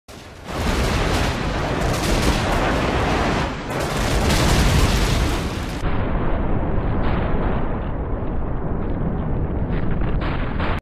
Low flying Airstrike?!?!
The easy way to avoid those overpowered airstrikes that always kill me when I'm on the other side of the map -_-